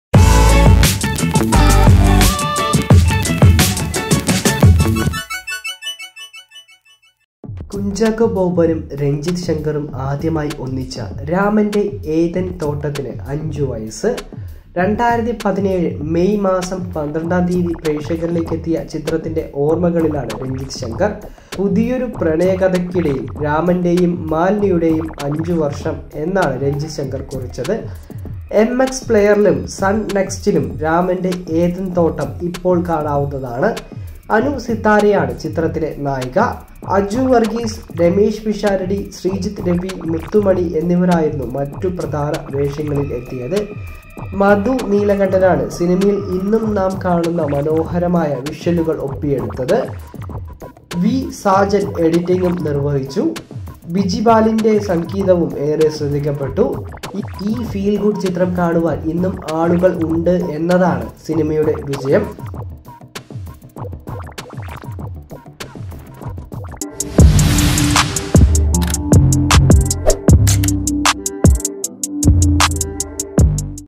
രാമന്റെ ഏദന്‍തോട്ടത്തിന് 5 വയസ്സ്
കുഞ്ചാക്കോ ബോബനും രഞ്ജിത്ത് ശങ്കറും ആദ്യമായി ഒന്നിച്ച രാമന്റെ ഏദന്‍തോട്ടത്തിന് 5 വയസ്സ്. 2017 മെയ് മാസം പന്ത്രണ്ടാം തീയതി പ്രേക്ഷകരിലേക്ക് എത്തിയ ചിത്രത്തിൻറെ ഓർമ്മകളിലാണ് രഞ്ജിത്ത് ശങ്കർ.